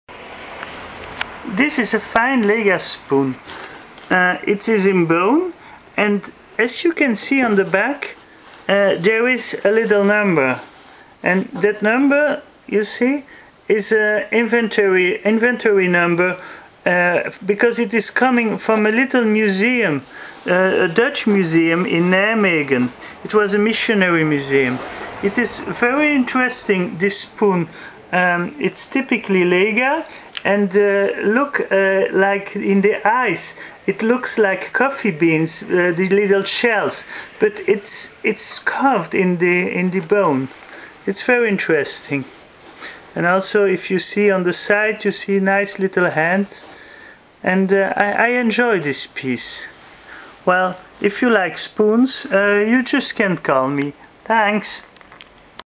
African art Lega bone spoon
A Lega bone spoon coming from the Stapelen Museum